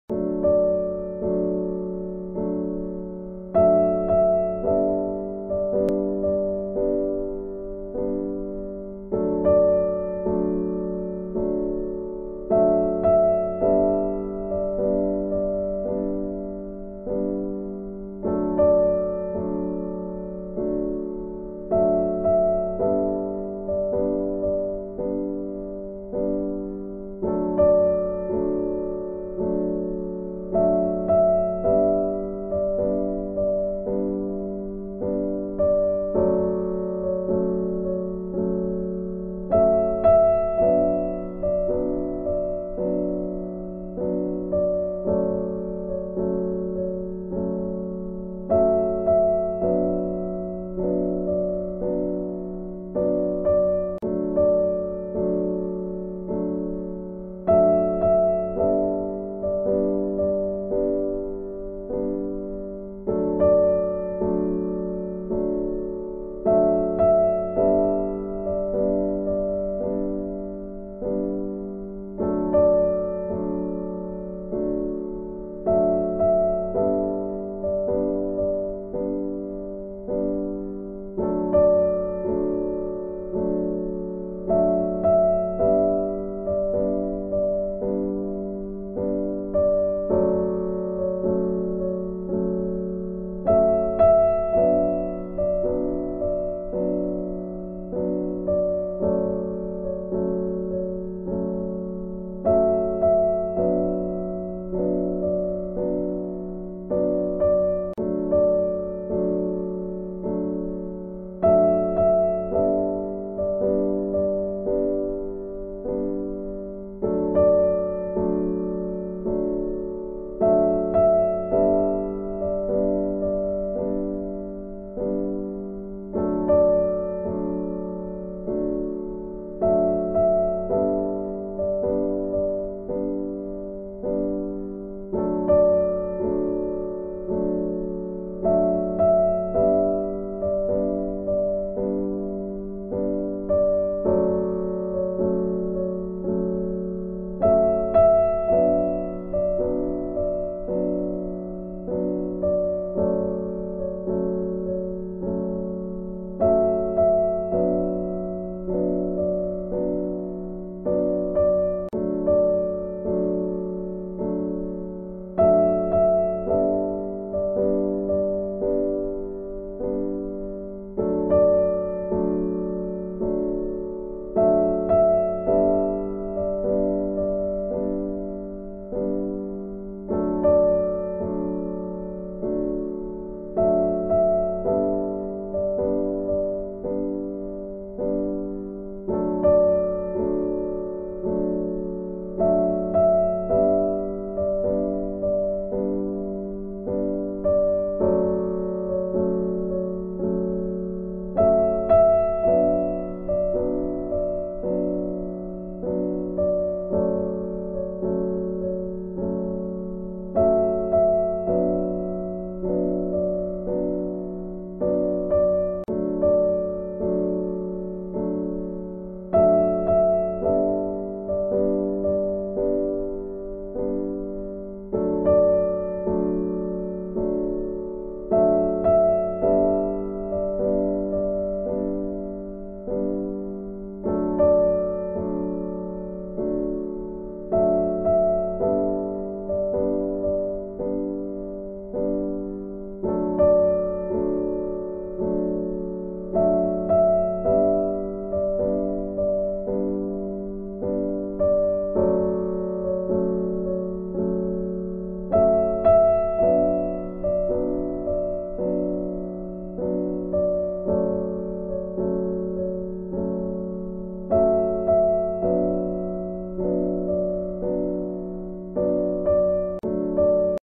anime drawing enjoy the comfortable music
Hey this a remake of my video all i changed is the background music the music is from inshot enjoy the musis